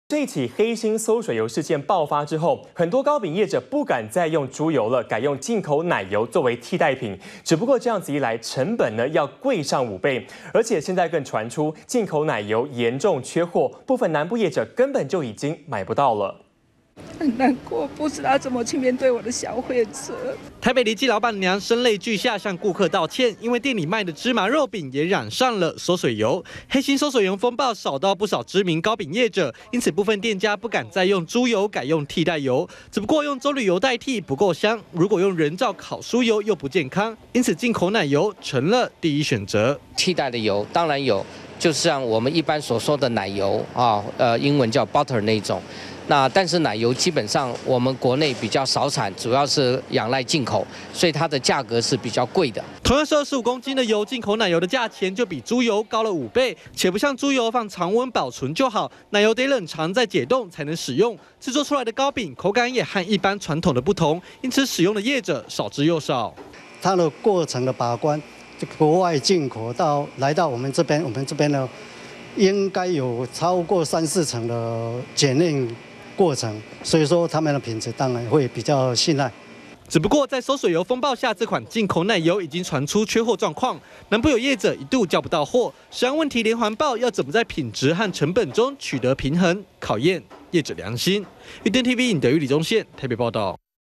【2014.09.08】餿水油風暴 替代品首選進口奶油 -udn tv
黑心餿水油事件爆發後，很多糕餅業者不敢再用豬油，改用進口奶油作為替代品，但成本貴上五倍，現在更傳出進口奶油嚴重缺貨，部份南部業者已經買不到。完整內容 , FB , Twitter , G+